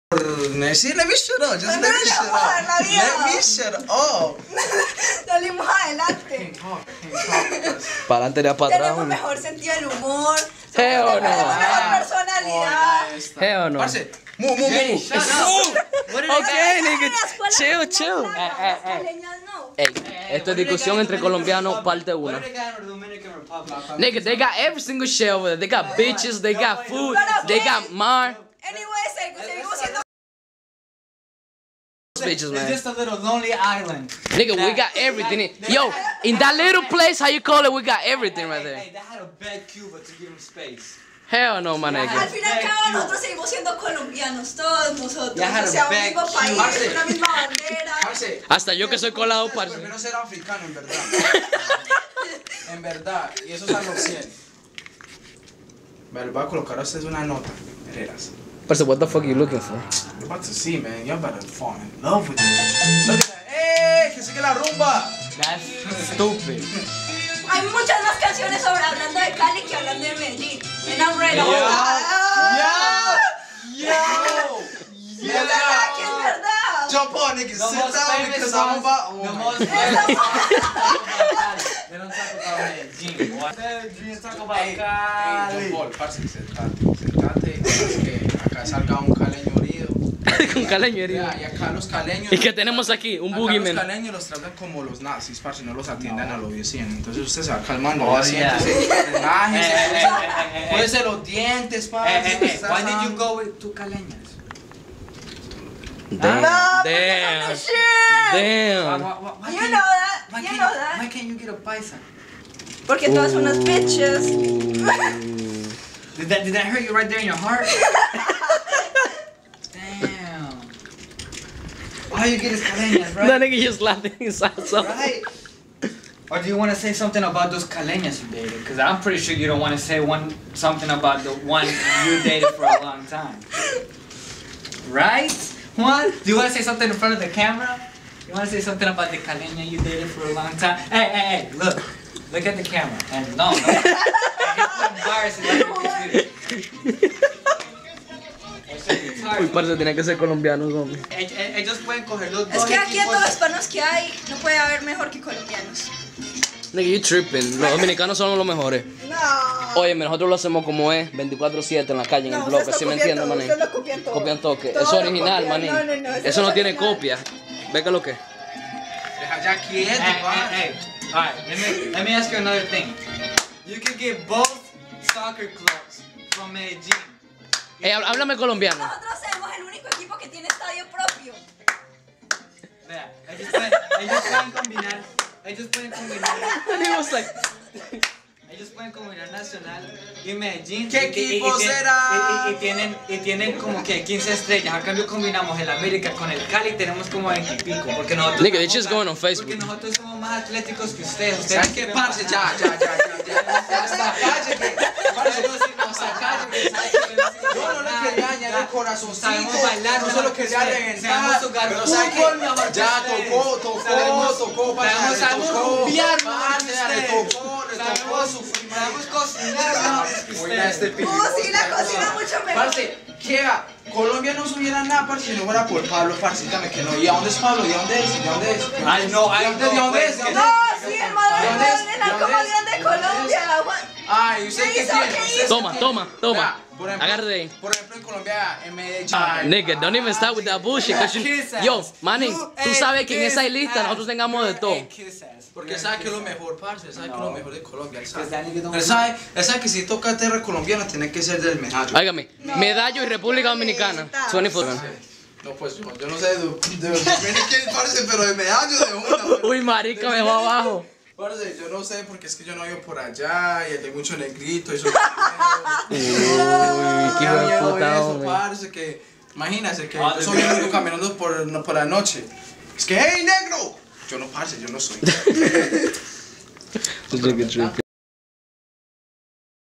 CALI VS. MEDELLIN...guerra entre colombianos!!.
aqui se discutio de to!! la pelea del año y la razon pk el boogie men no hay salido con ninguna paisa si no caleñas!! enjoy